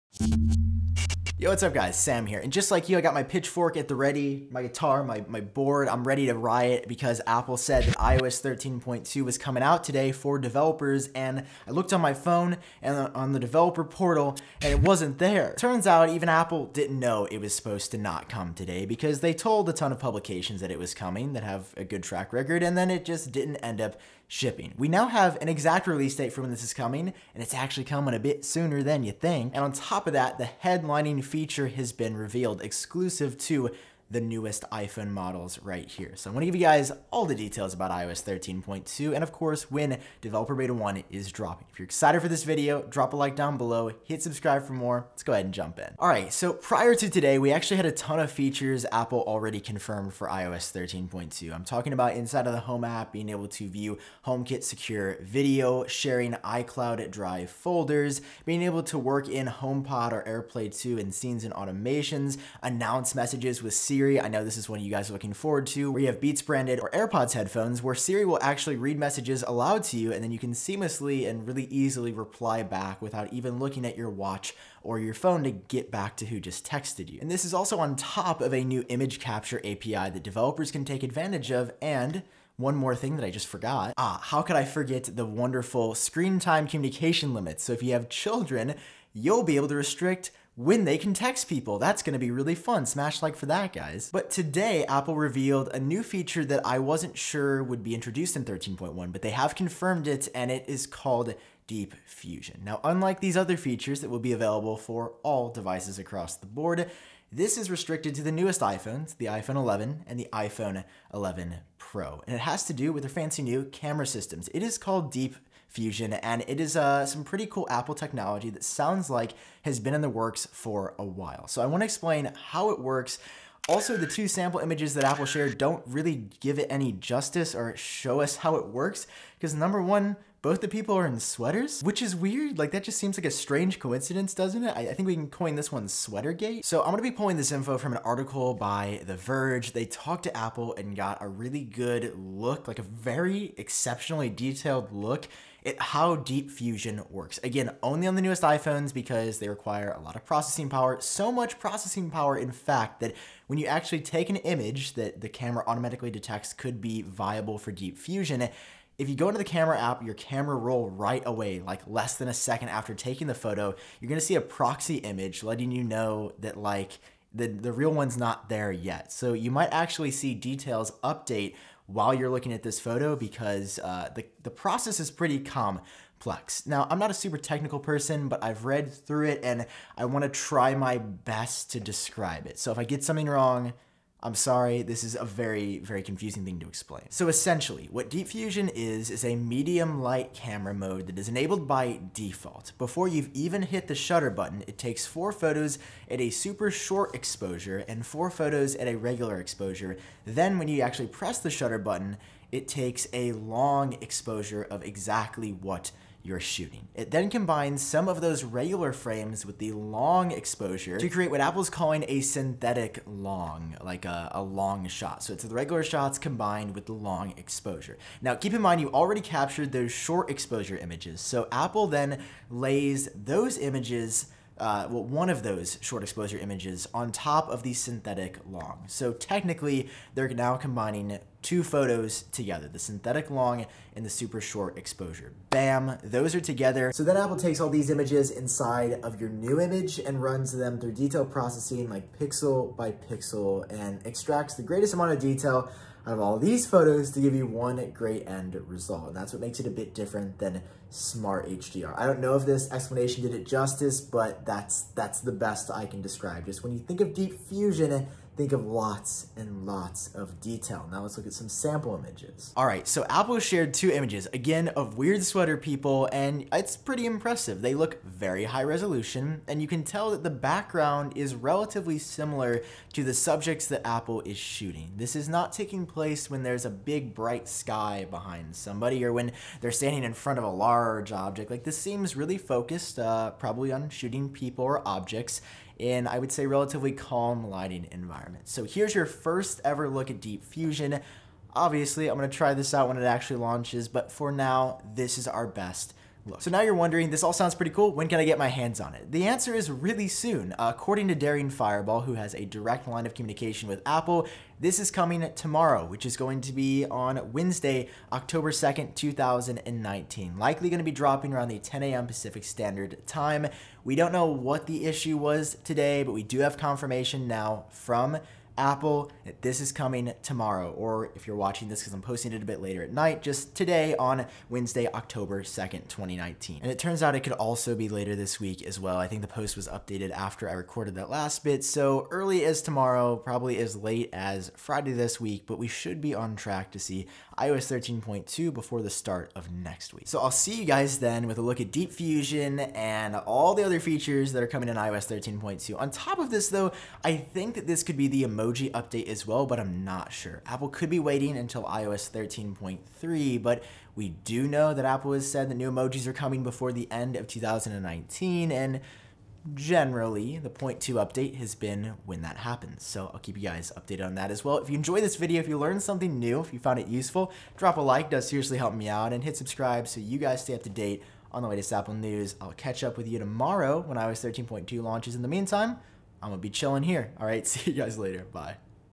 iOS 13.2 Beta 1 Coming Today! Deep Fusion & More
The first iOS 13.2 beta is releasing to developers as soon as tomorrow (October 2) with a new feature for iPhone 11 and 11 Pro called Deep Fusion: it combines a number of images to create one, ultra-detailed image in a sleek computational photography process. iOS 13.2 is also set to bring Announce Messages with Siri, HomeKit Secure Video, iCloud Drive folder sharing and more. Will you be hopping on the developer or public beta when it releases this week?